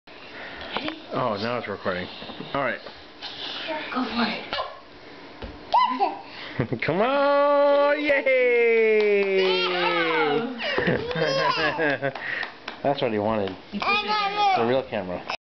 Curtis walking
We were successful to get him walking on film.  Funny to see what we had to bribe him with  :)